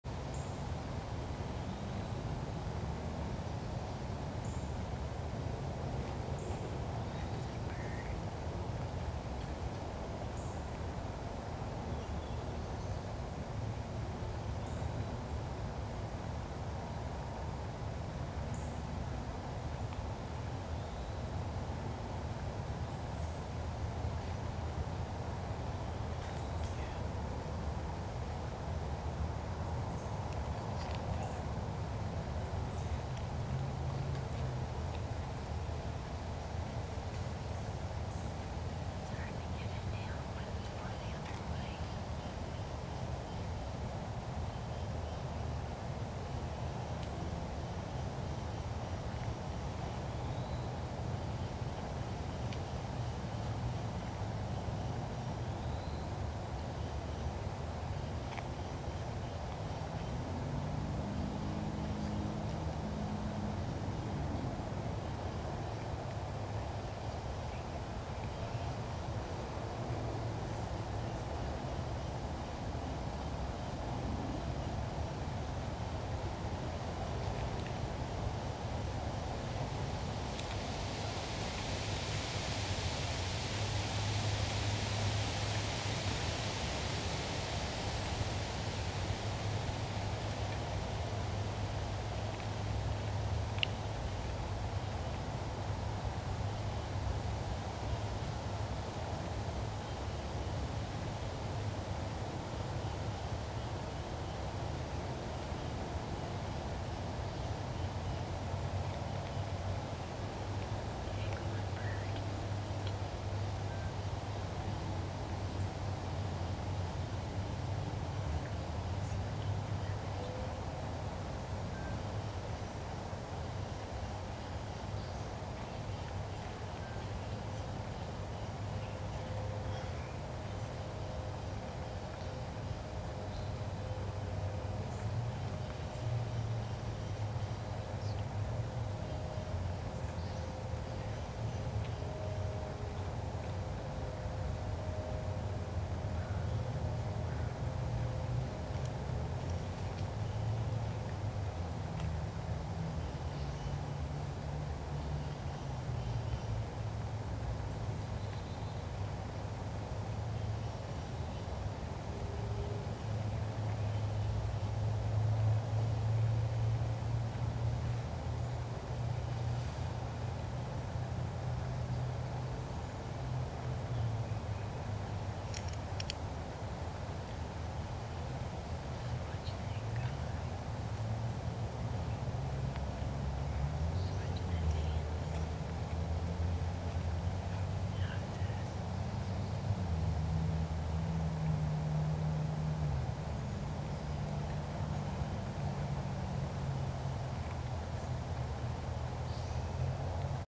Blacklick 15; Barred owlet tries to swallow bird
Barred owlet tries to swallow part of a bird, and can't quite manage it. Owlets are about 32 days old. Number 15 of 53 videos of this nest. They are in chronological order. Taken May 17, 2011, at Blacklick Woods Metro Park, Metro Parks of central Ohio. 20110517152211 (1)